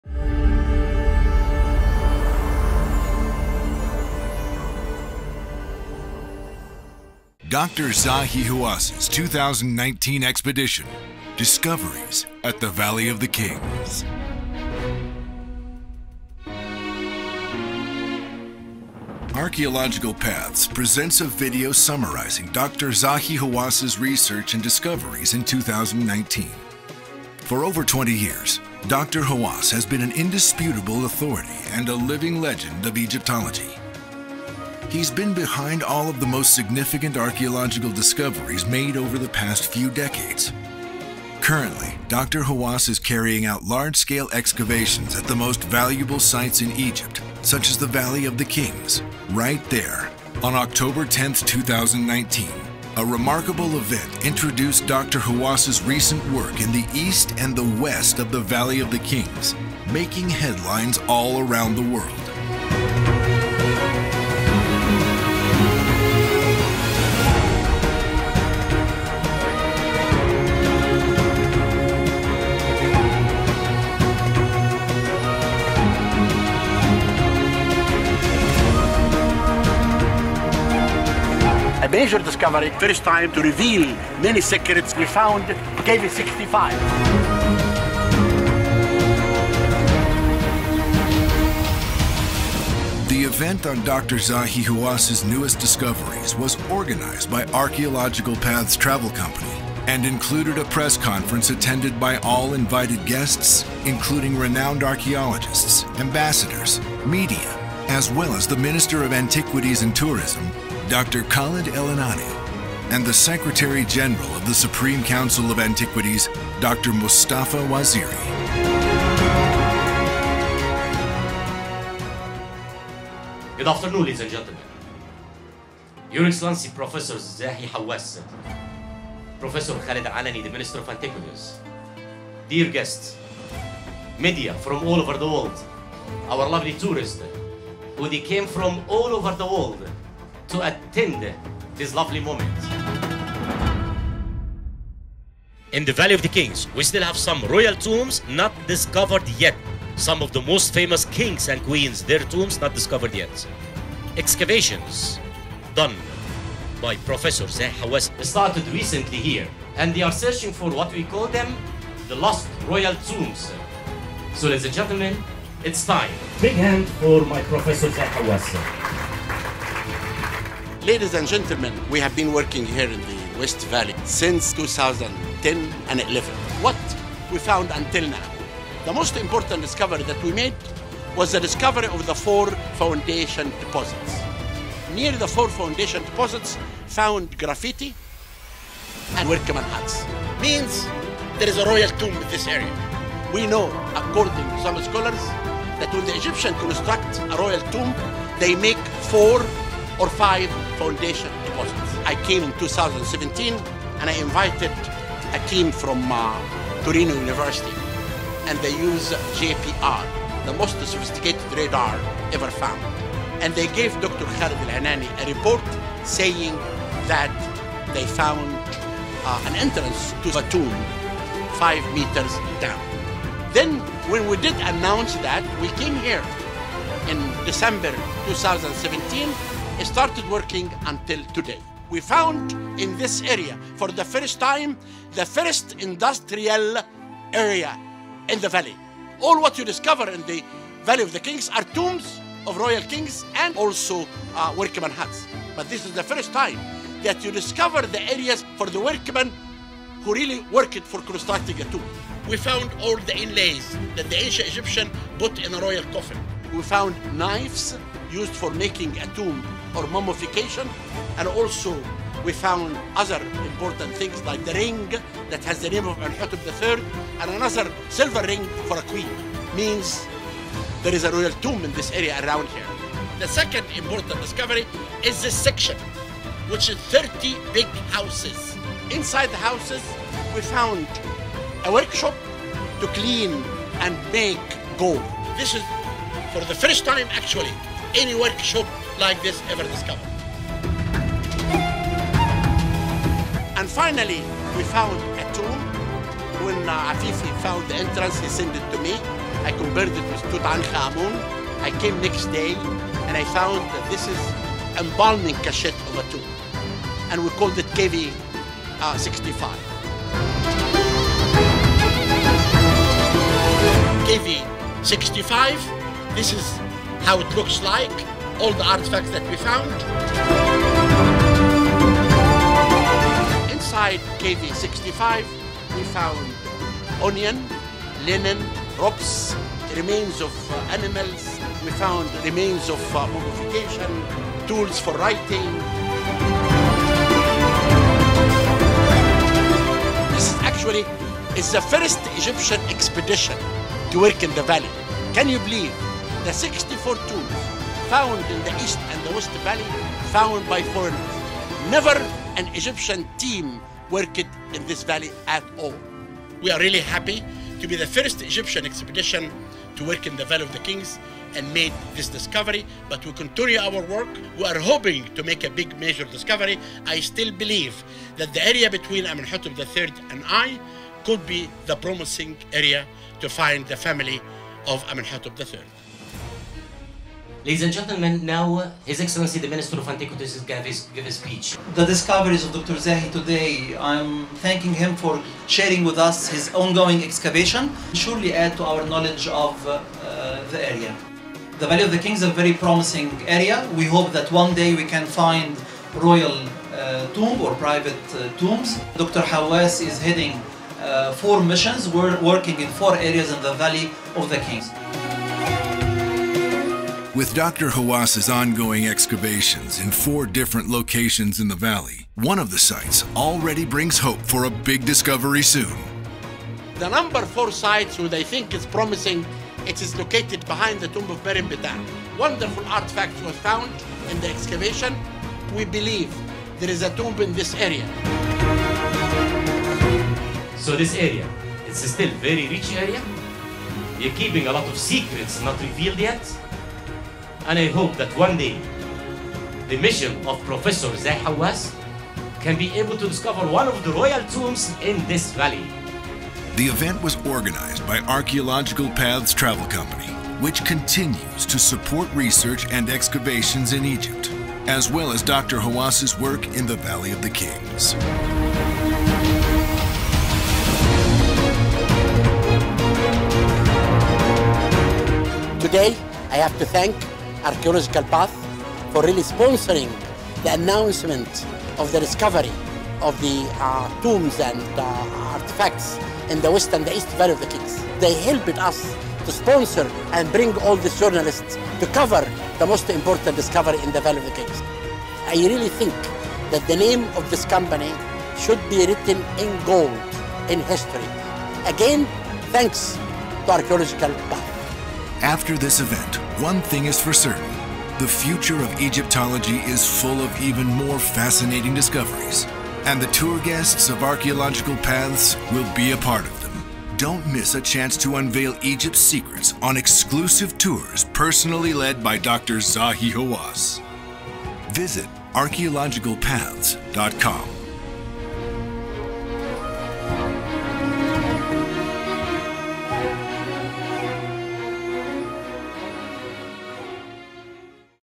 Dr. Zahi Hawass’ 2019 EXPEDITION – Discoveries at the Valley of the Kings
Archaeological Paths presents a video summarizing Dr. Zahi Hawass’ research and discoveries in 2019. For over 20 years, Dr. Hawass has been an indisputable authority and a living legend of Egyptology. He’s been behind all the most significant archaeological discoveries made over last few decades. Currently, Dr. Hawass is carrying out large-scale excavations at the most valuable sites in Egypt such as the Valley of the Kings. Right there, on October 10th, 2019, a remarkable event introduced Dr. Hawass’ recent work in the East and the West of the Valley of the Kings, making headlines all around the world.